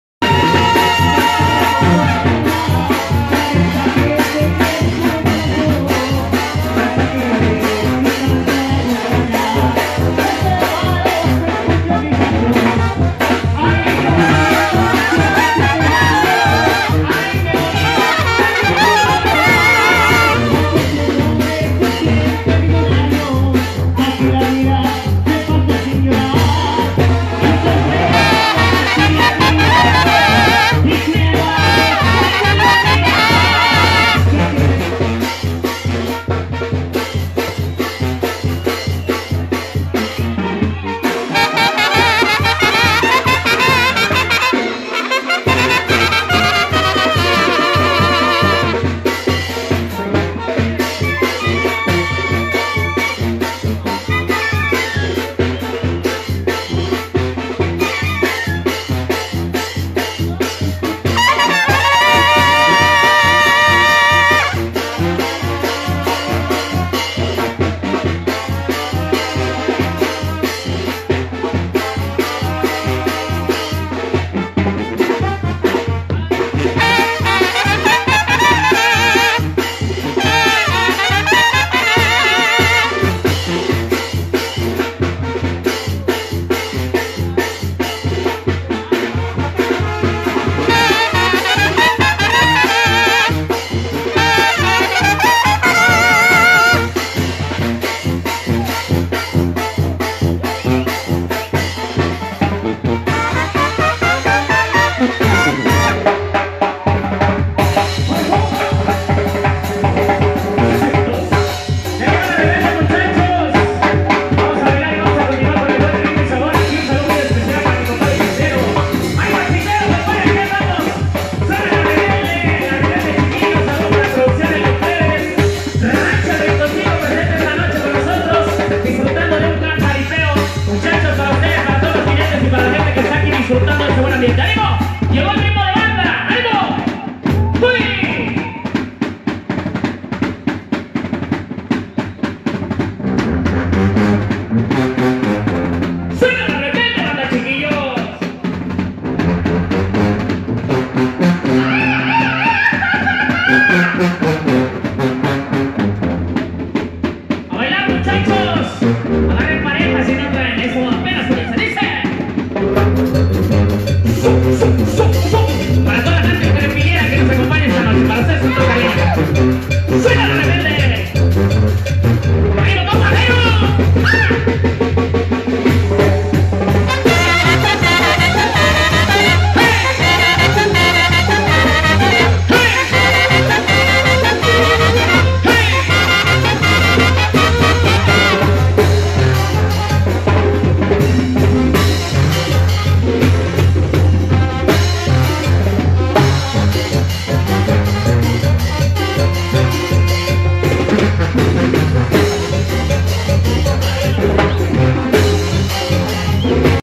LA REBELDE BANDA CHIQUILLOS EN VIVO PRESENTES EN PLAZA LOS PLEBES 31 De Agosto
INFORMES Y VENTAS PRODUCCIONES LOS PLEBES